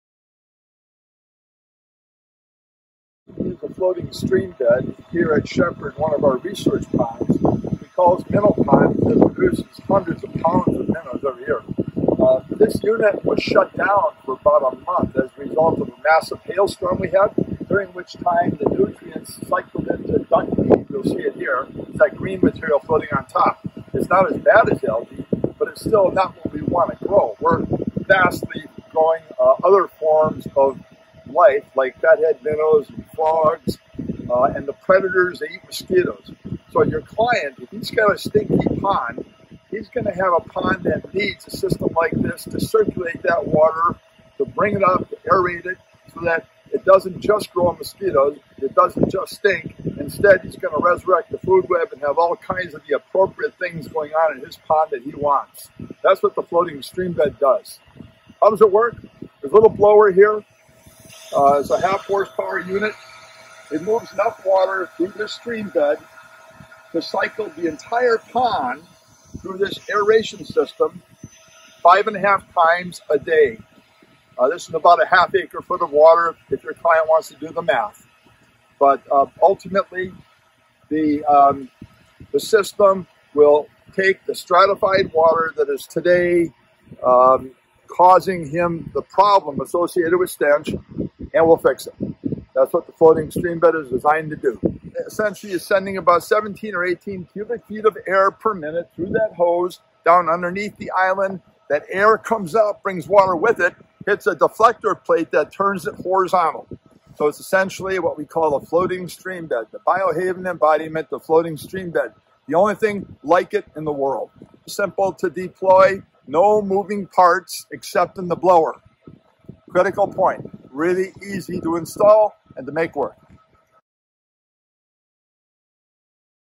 BioHaven StreamBed Solves Harmful Algae Bloom (HAB) issues
The BioHaven StreamBed is our model of BioHaven Floating Island that incorporates aeration, to provide surface area and circulation in one solution.  It is very simple to install and operate.  It has a "stream" channel built in to the top that is filled with flowing water which is lifted from the pond by an airlift pump. The stream channel can be accessorized with other products and materials such as Biochar bags, which adsorb phosphorus.  If your pond or lake has a Harmful Algae Bloom, the BioHaven StreamBed is at your service.